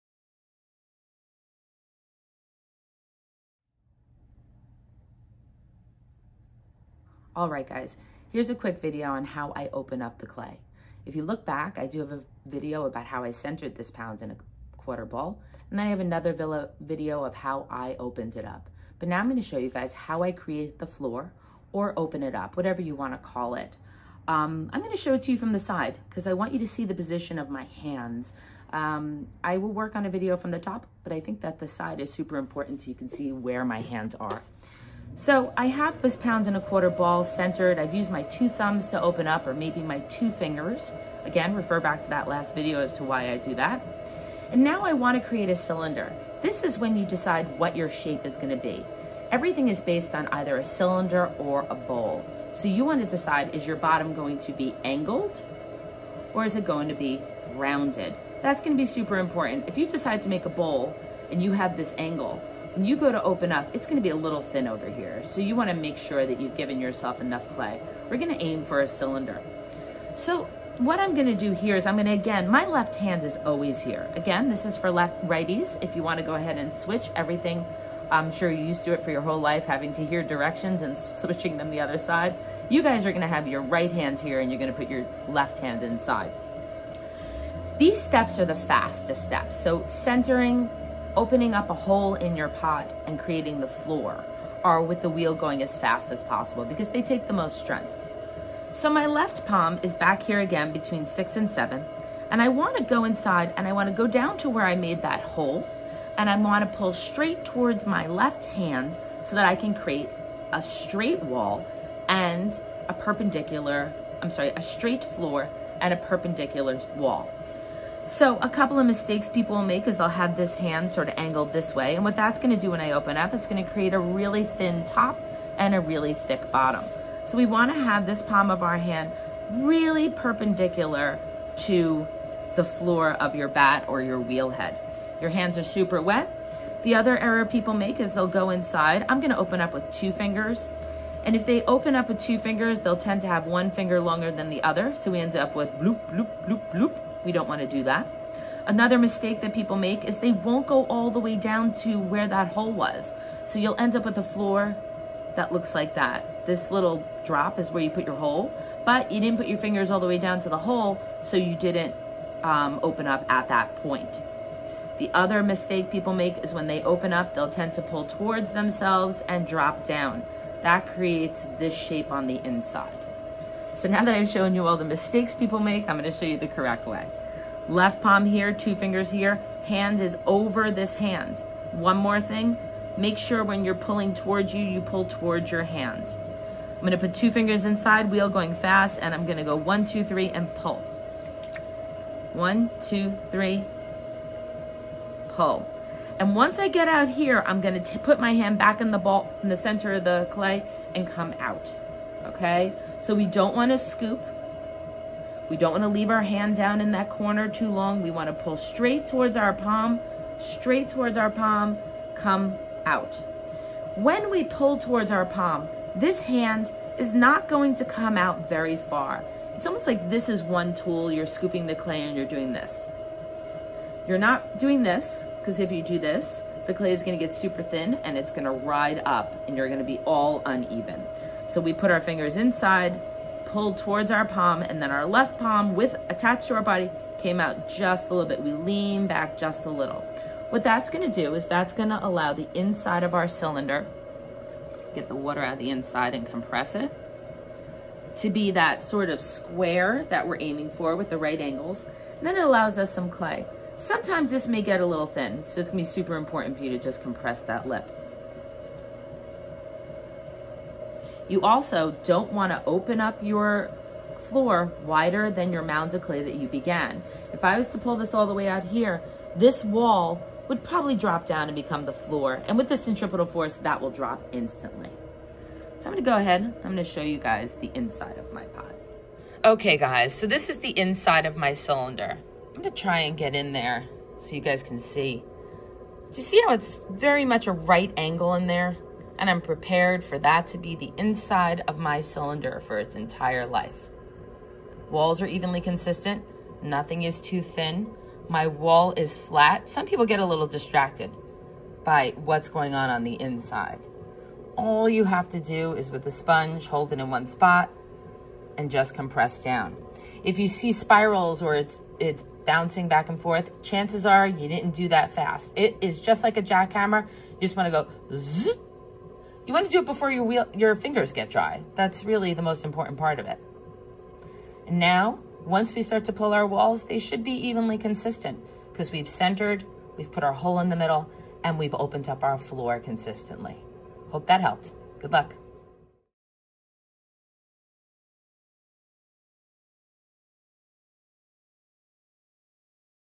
The Basics: Creating the Floor of a Pot
Let's break it down! Here is the third of a series of videos breaking down every step of throwing a small cylinder. This pottery video addresses how to create the floor of a  1.25lb cylinder The floor determines what the final size and shape will be. Here are some do's and don'ts constructing a perfect cylinder on a potters wheel.